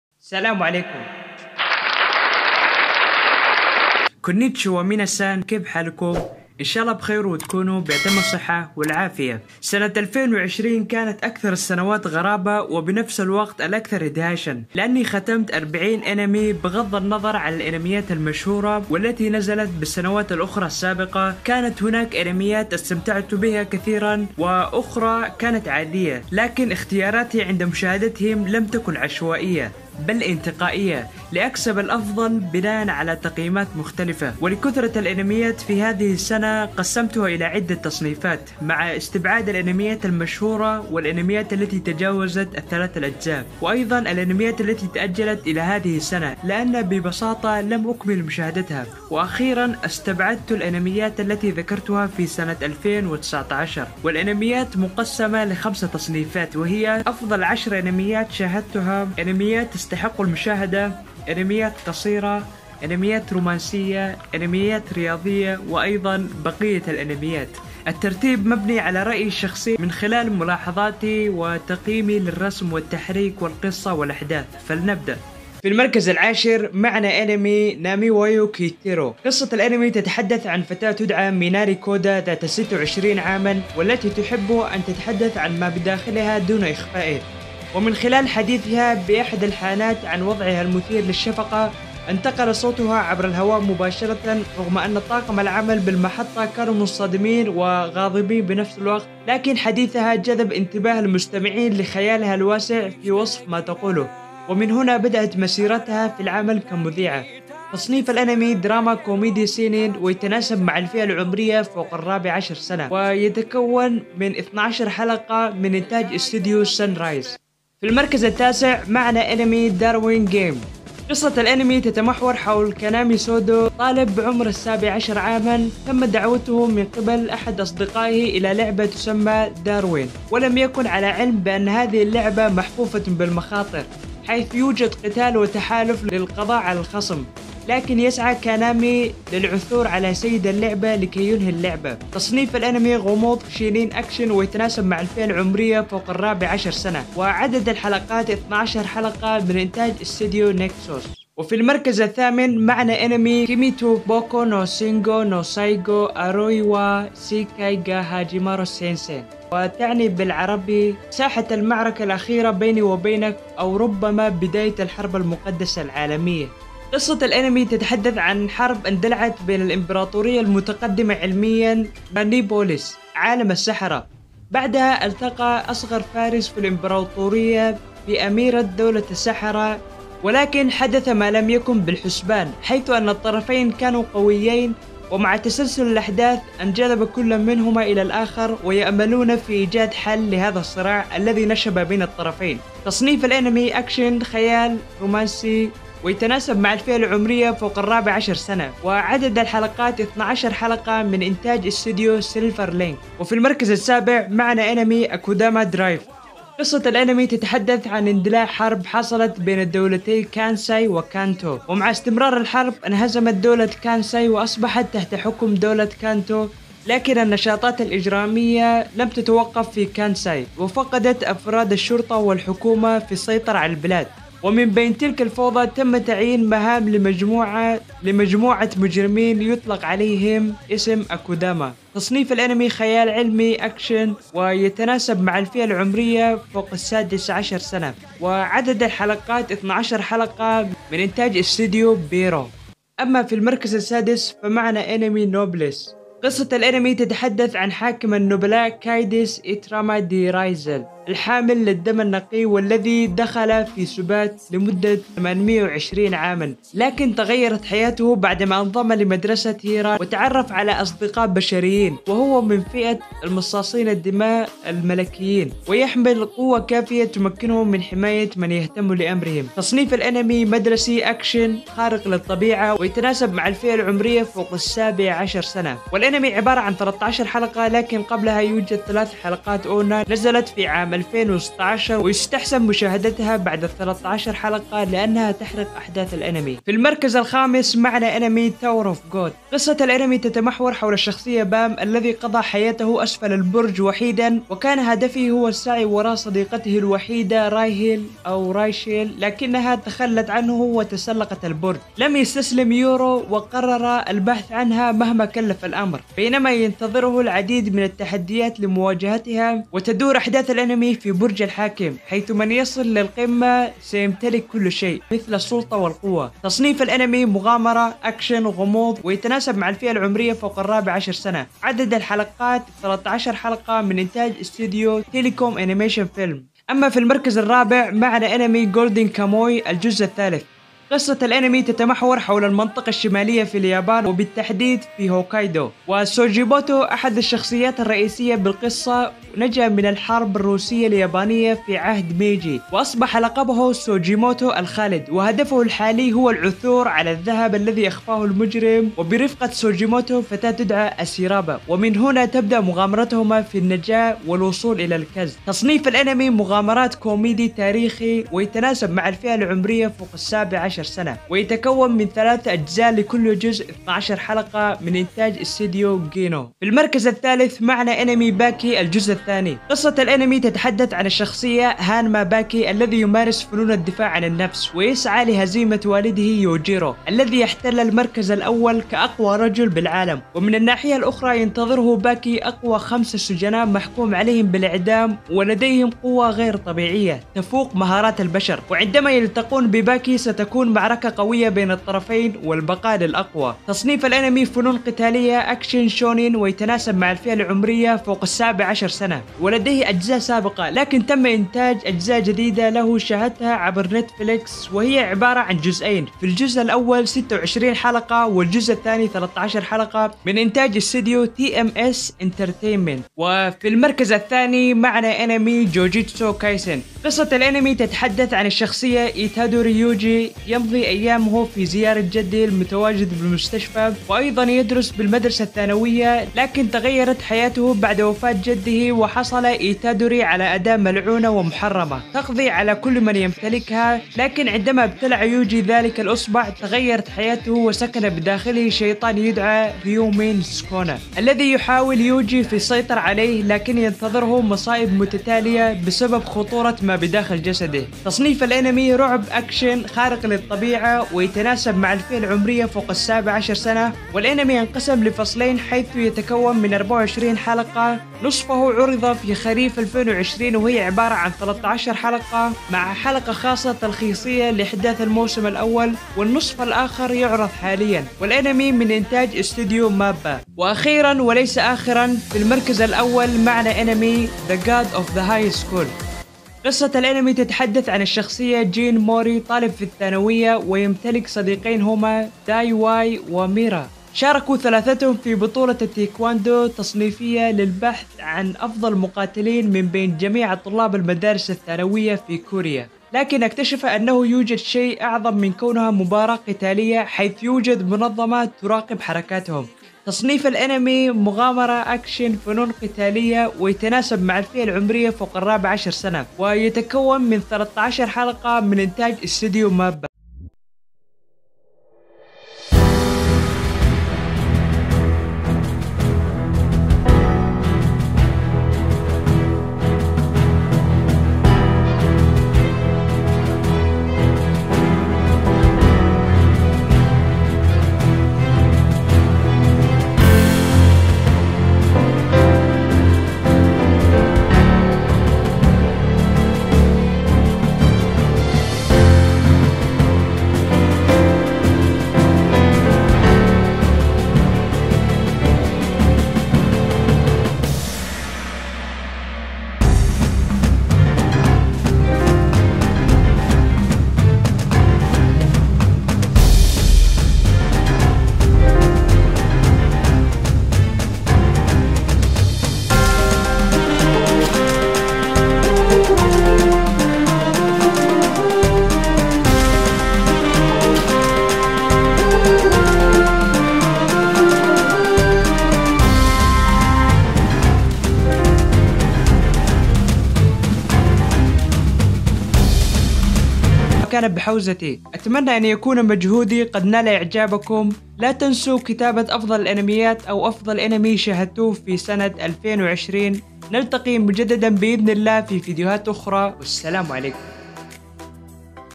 أفضل الأنميات التي شاهدتها في عام 2020 | لا تفوتك 🔥
- سنة 2020 كانت أكثر السنوات غرابه وبنفس الوقت الأكثر إدهاشاً لإني ختمت 35 أنمي دون ذكر الأنميات المشهورة والأنميات التي نزلت بالسنوات السابقه مثل سلسلة fate zero وغيرها، كانت هناك أنميات استمتعت فيها كثيراً في ٢٠٢٠ وأخرى كانت عاديه لكن إختياراتي عند مشاهدتهم لم تكن عشوائيه بل بإنتقائيه لأكسب الأفضل بناءً على تقييمات مختلفه

00:00 ¦ المقدمه
01:16 ¦ المركز العاشر
01:58 ¦ المركز التاسع
02:31 ¦ المركز الثامن
03:23 ¦ المركز السابع
04:03 ¦ المركز السادس
04:47 ¦ المركز الخامس
05:27 ¦ المركز الرابع
06:06 ¦ المركز الثالث
06:53 ¦ المركز الثاني
07:50 ¦ المركز الأول
08:30 ¦ أنميات تستحق المشاهده
08:57 ¦ أنميات رومانسية
09:10 ¦ أنميات قصيره
09:17 ¦ أنميات رياضيه
09:27 ¦ بقية الأنميات اللي ما لقيت لها تصنيف
10:02 ¦ الخاتمة

" قد تختلف معي في ترتيب هذه الأنميات لكنها تضل ترتيبي الشخصي، أكتبوا ترتيب قائمتكم للأنميات أو أفضل أنمي شاهدتموه في سنة 2020 بالأسفل ..

- شكرا لـ تواجدكم، والسلام عليكم .